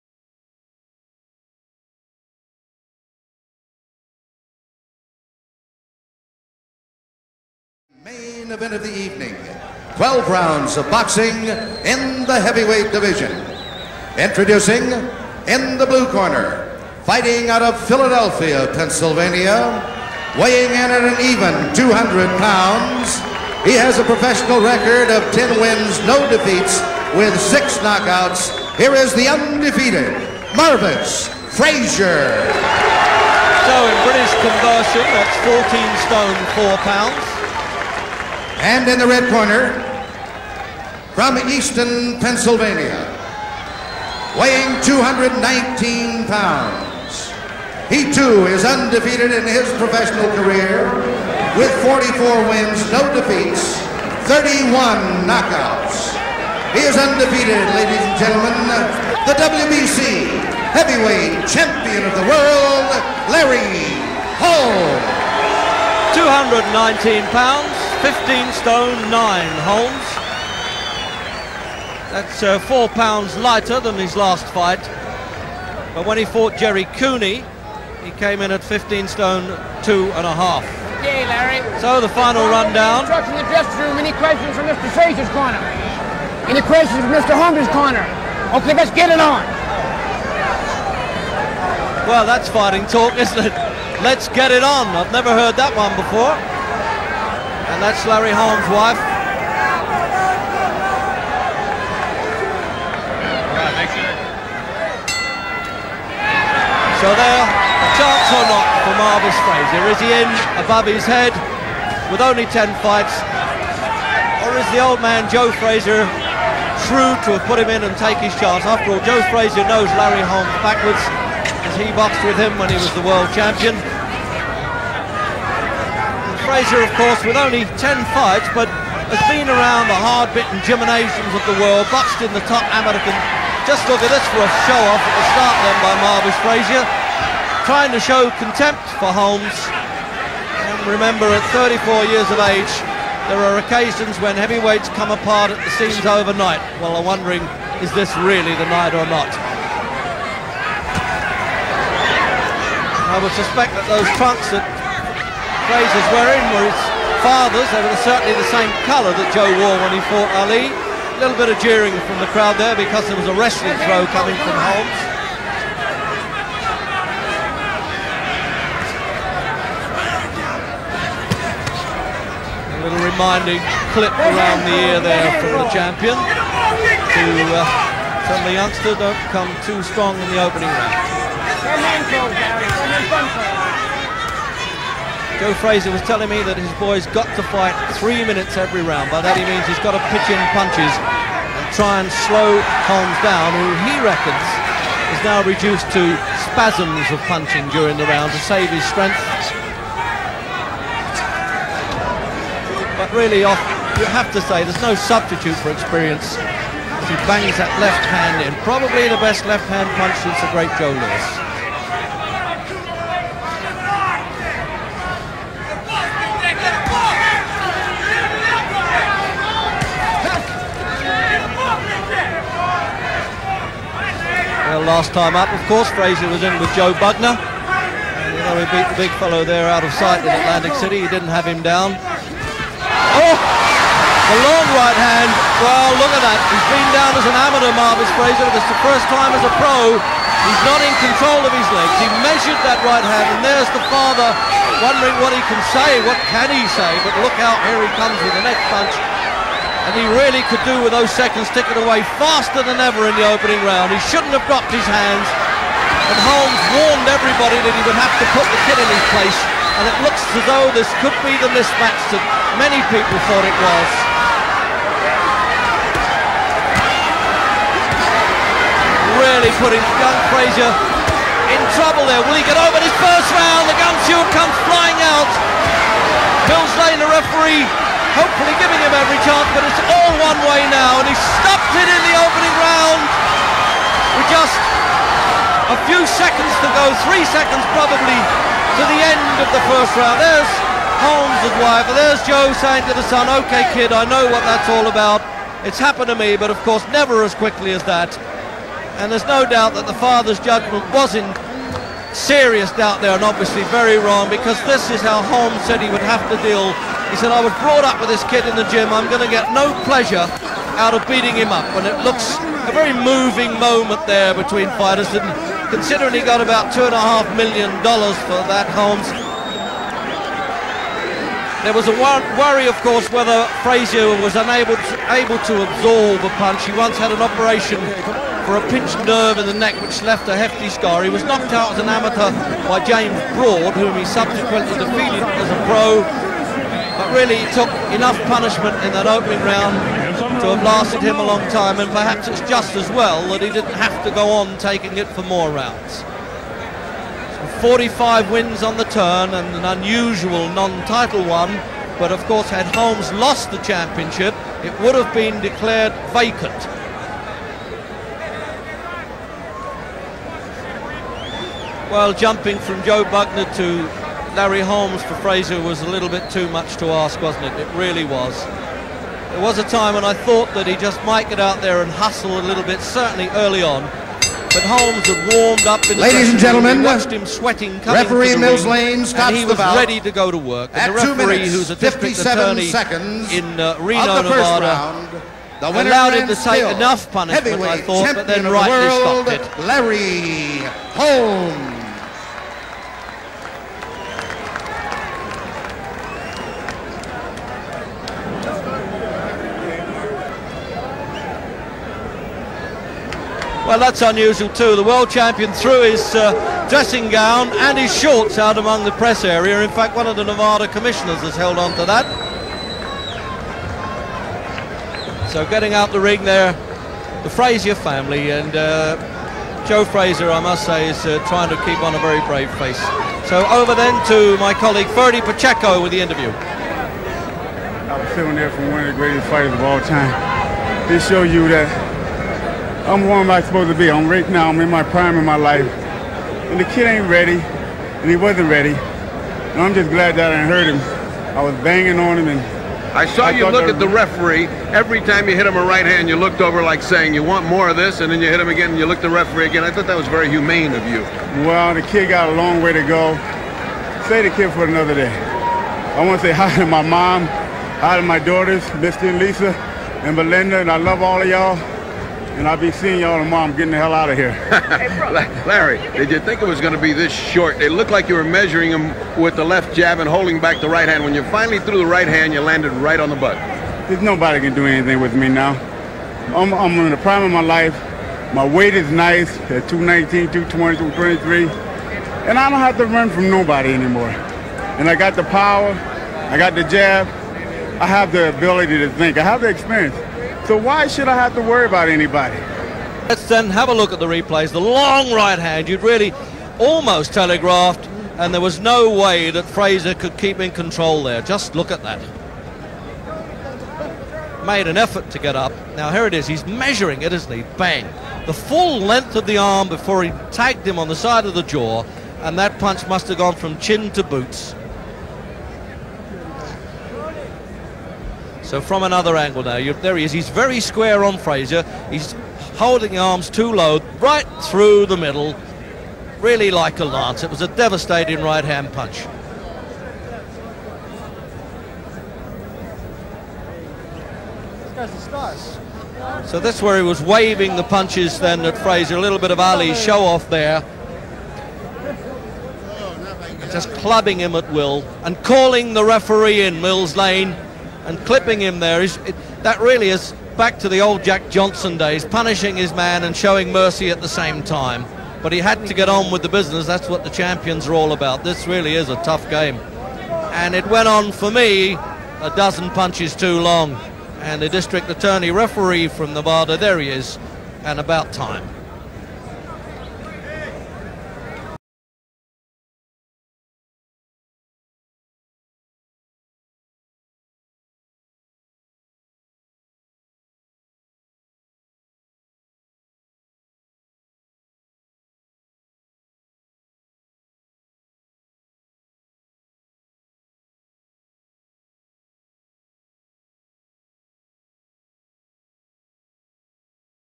Larry Holmes vs Marvis Frazier (in high quality)
WBC world heavyweight title fight - 1983.